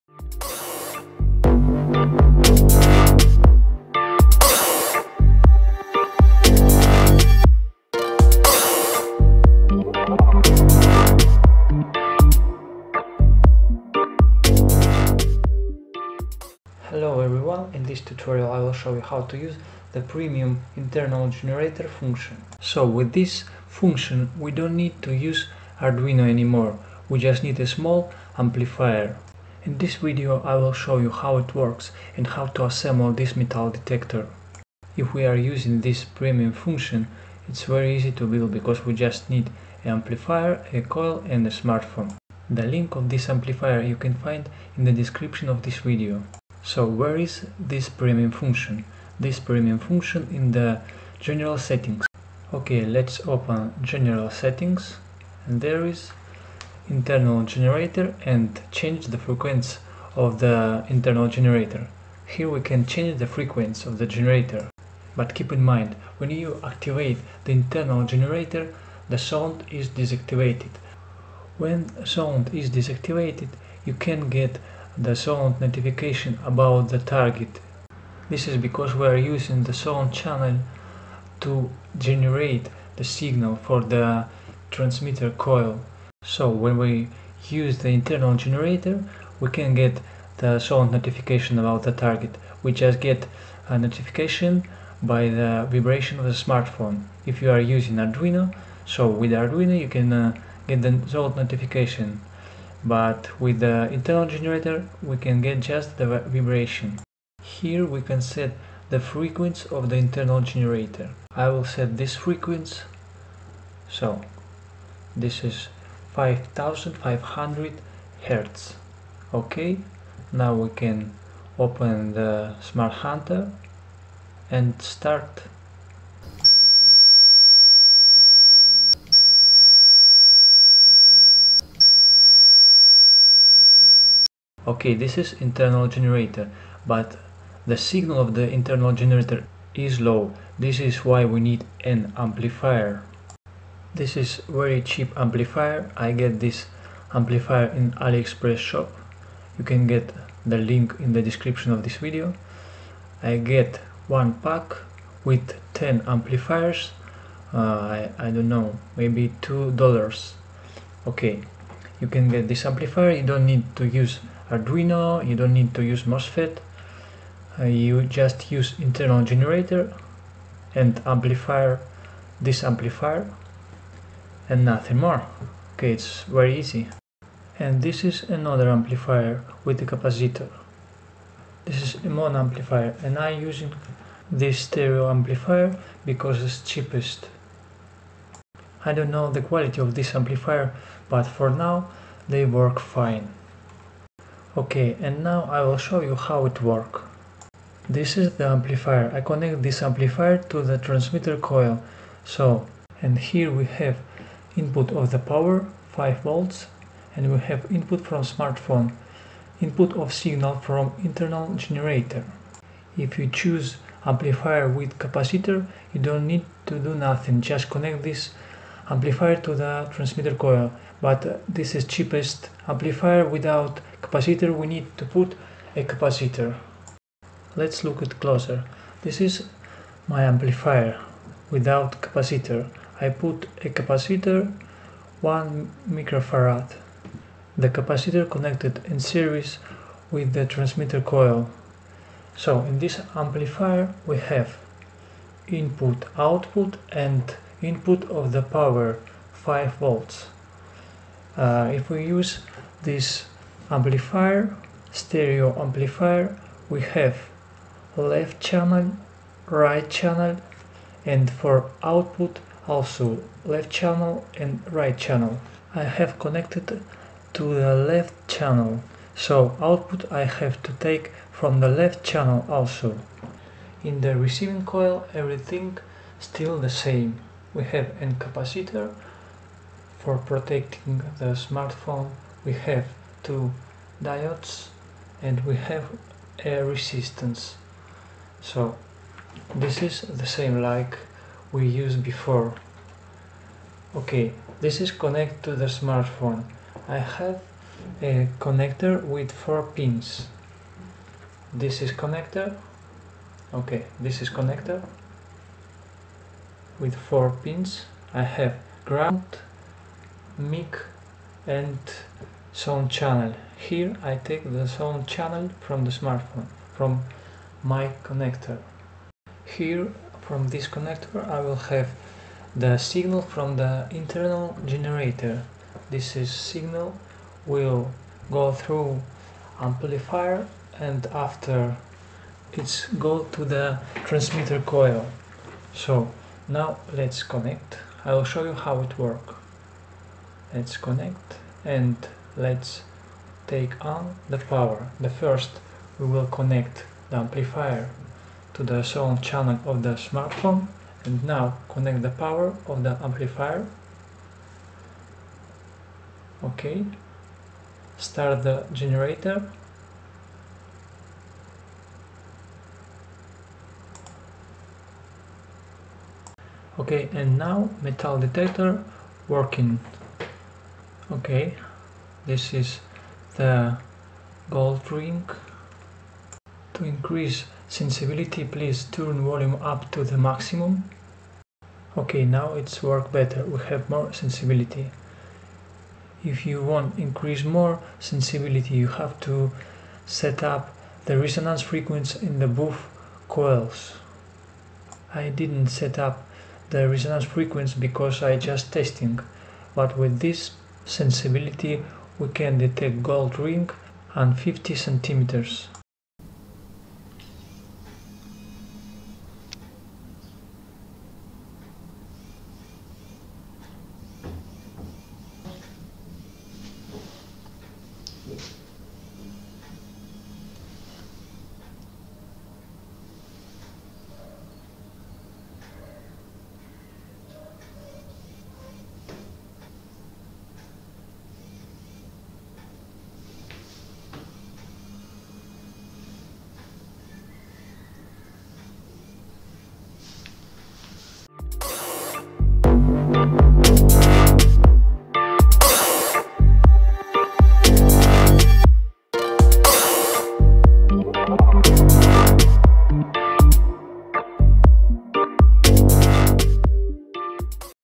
Premium functions on MD "Smart Hunter"
In this tutorial i will show you how to use premium function "Internal generator".
This function help us to build this MD easier!